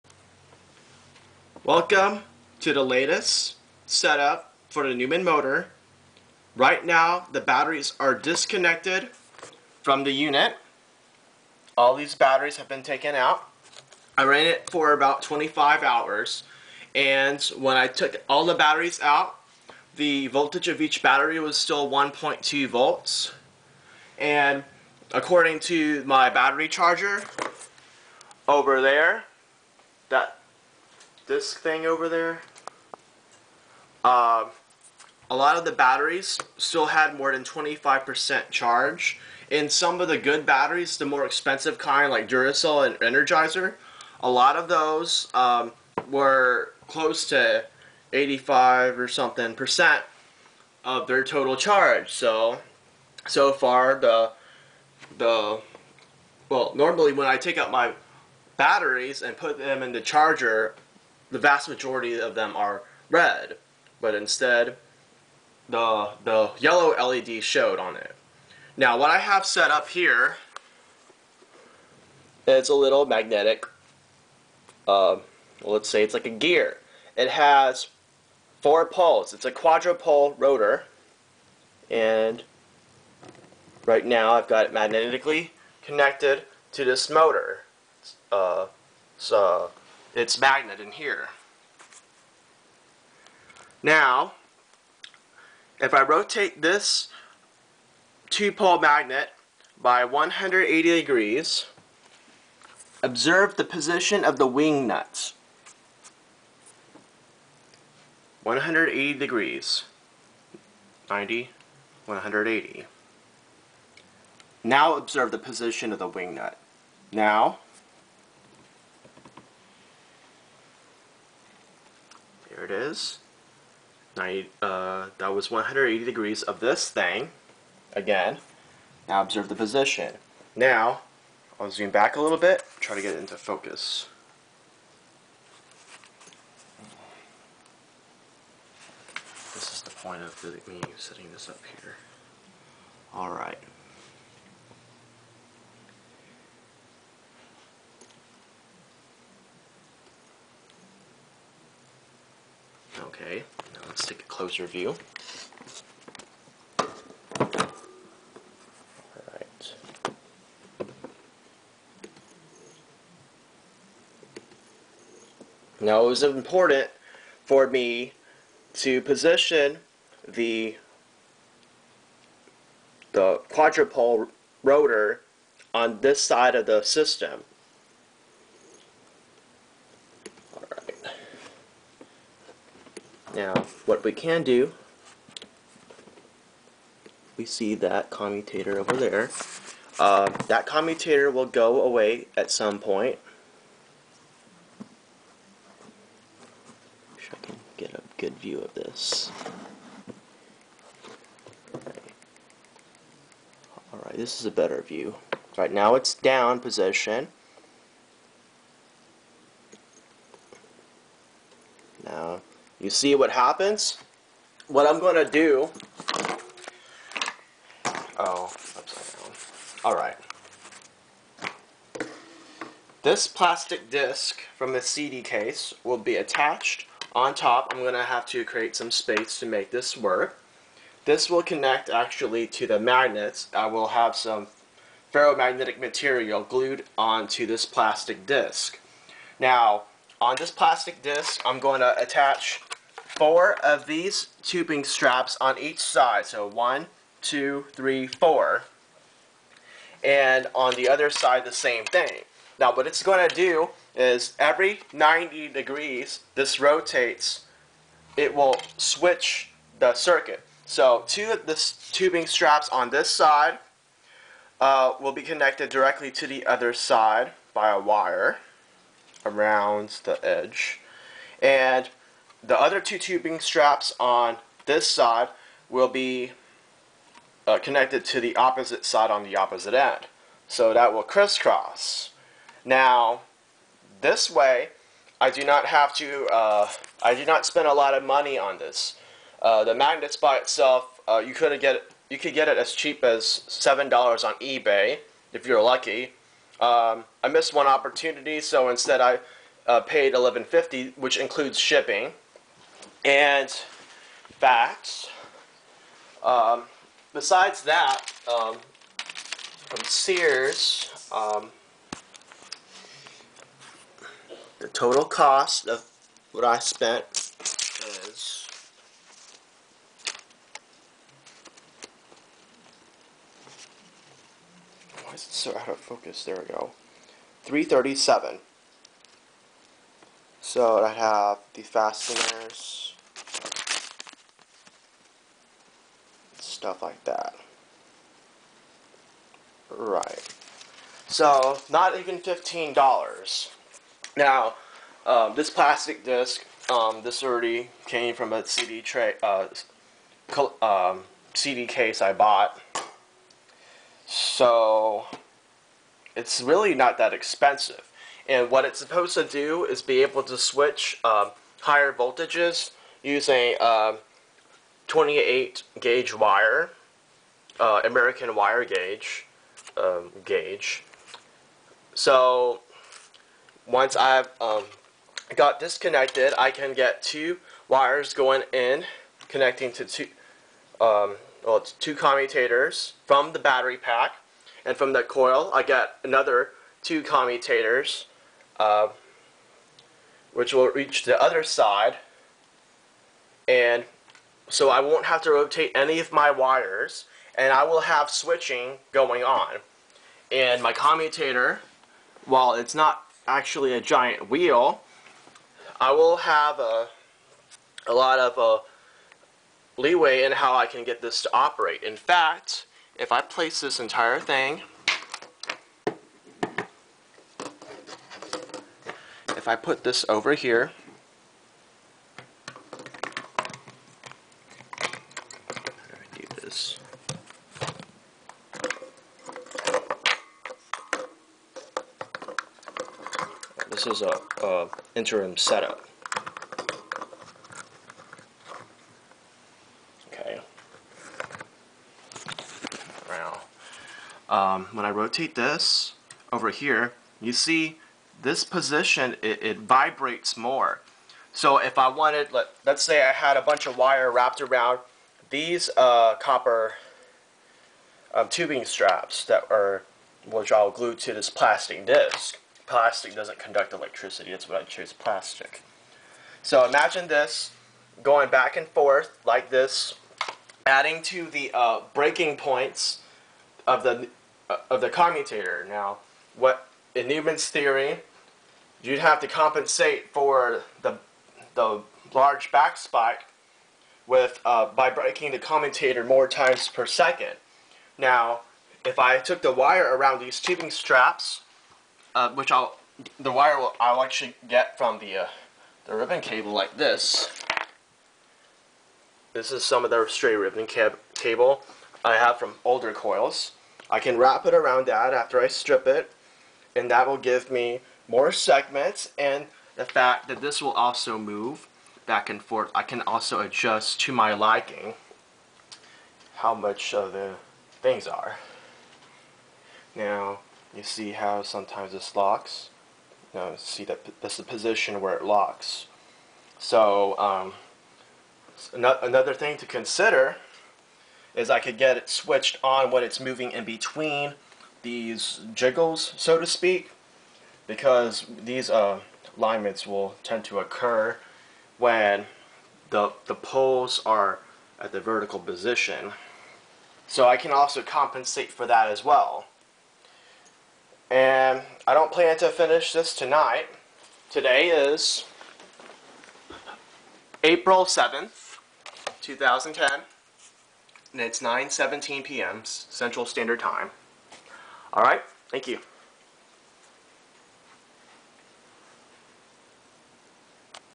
Newman Motor: Reversing-Switch plan uses 2-to1 Magnetic Gearing (at a distance)
(: 00870 = days since my first newman motor :)

New Plan: I will use a system of 4 reed switches, but I will still use the magnets because the reed switches I am going to use are not polarity dependent. I will rearrange the permanent magnets in two separate stacks at opposite ends of the bolt shaft, each separated by 180 degrees. Each of the stacks will have the same polarity facing outwards. Example:

S______
N shaft S
______N
2 reeds 2

Reed switch:
1) To connect the + side of the battery pack to the A side of the coil
2) To connect the B side of the coil to the - side of the battery pack
3) To connect the + side of the battery pack to the B side of the coil
4) To connect the A side of the coil to the - side of the battery pack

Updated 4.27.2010

Old plan: double the torque, I plan to build a simplified relay switch that will reverse the current by using a quadrupole permanent magnetic rotor connected to a wheel that will contain the switching units. This should bring up the RPM by about 40% (=sqrt(2)-1) while only doubling the power input. Fan laws dictate this would be about a 40% increase in efficiency. 40% faster in this case would take it over 150 rpm. If having it on during both sides, not just one side, improves mechanical efficiency, then the results will improve by more than 40%.

Buying batteries would not be the way to do it because I would need to get 370 volts to get the speed up by the same amount, and also, the construction of my machine is such that efficiency does not increase with the voltage but remains flat until about 100mA are drawn, after which it becomes less efficient. Finally, my wires are only rated up to 300V. So I will get a relay!

After that, to go faster, I would probably buy two more coils and connect two sets of four coils in parallel so I can still use less than 250V. I wanted to get this fan to spin at 200RPM last year, but hopefully I will finish that by the end of this year. As it is right now, the output can be compared to my ceiling fan, and at 200RPM it would be 5 times more powerful.